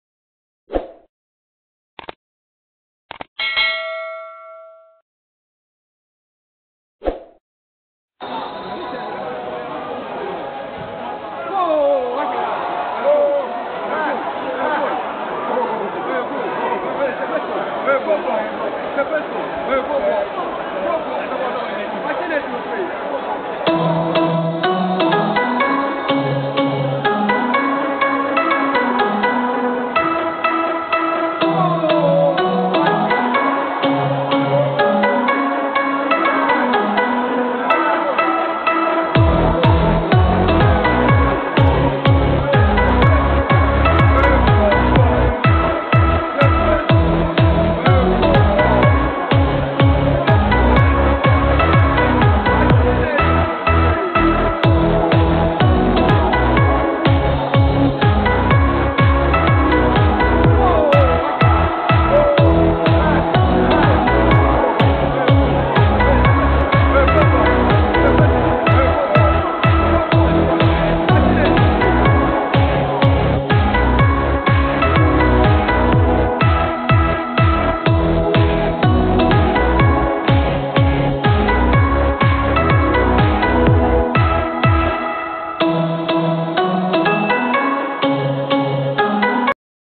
Why the Ghana Premier League needs a VAR. Elmina Sharks vs Inter Allies
How on earth did the referee not see the ball cross the line?